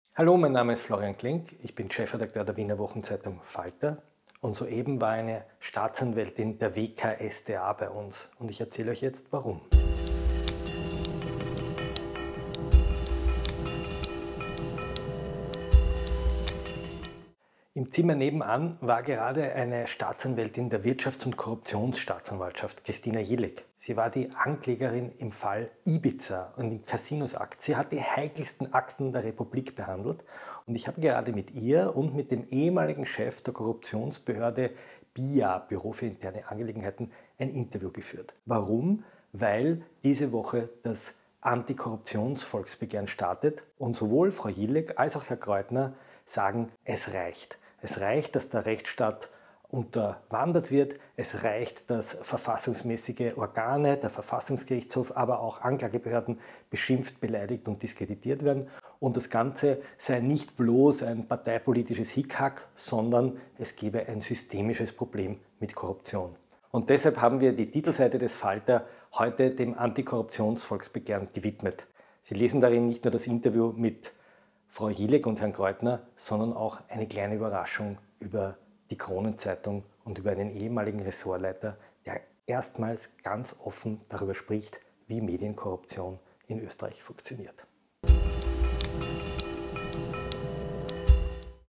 Florian Klenk über Korruption und Expertinnen, denen es reicht
Florian Klenk hat die frühere Staatsanwältin Christina Jilek und den ehemaligen Chef der BIA Martin Kreutner interviewt.

Beide unterstützen das Antikorruptionsvolksbegehren und sagen: Es reicht!
Es reicht, dass der Rechtsstaat unterwandert wird, und es reicht dass Anklagebehörden beschimpft, beleidigt und diskreditiert werden.

Ausserdem spricht zum ersten Mal ein ehemaliger Ressortleiter der Kronen Zeitung über Medienkorruption. Hier finden Sie mehr dazu: